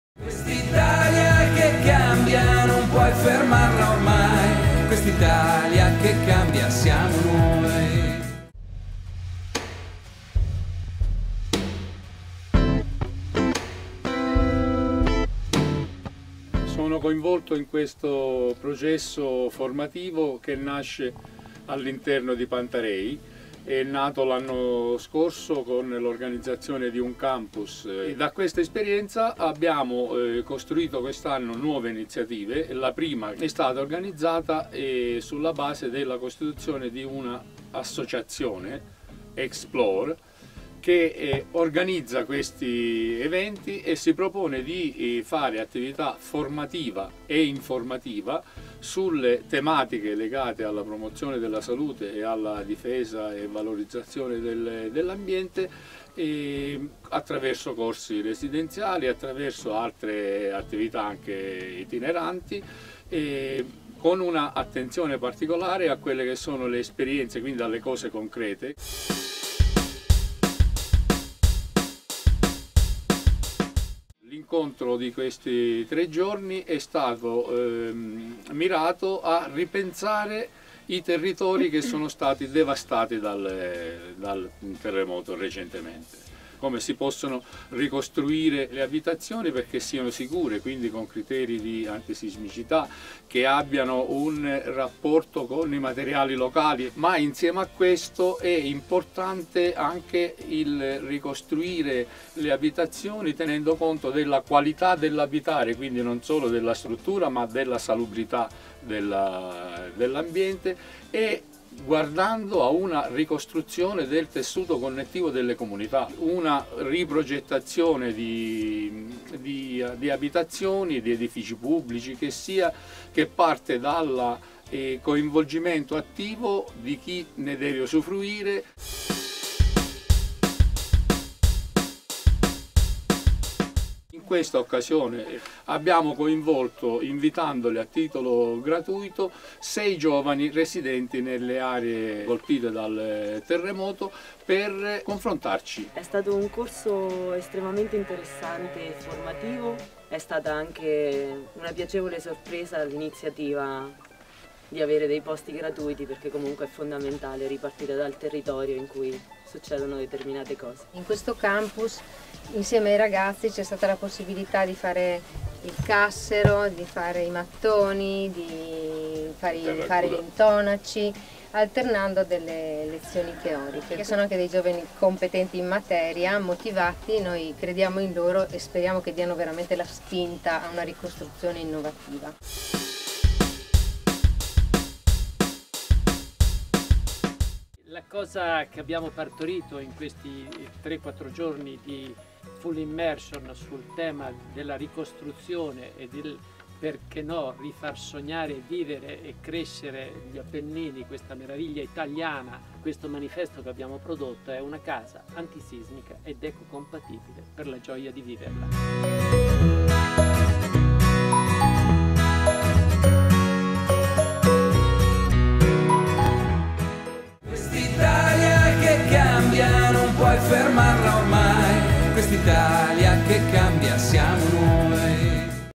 L'Umbria dopo il terremoto: una ricostruzione sostenibile e partecipata
Dal 2 al 5 marzo 2017 si è tenuto a Panta Rei, in Umbria, il corso "Ripensare le case: autocostruzione, antisismicità e compatibilità ambientale" organizzato dall'Associazione Explore, allo scopo di mettere l’esperienza ed il know-how, maturati in 20 anni nella costruzione di Panta Rei, a disposizione dei territori colpiti dal terremoto ed i fornire così' uno strumento alternativo alle comunità locali per la progettazione e la costruzione partecipate di strutture anti-sismiche, a basso impatto ambientale, ed eco-compatibili.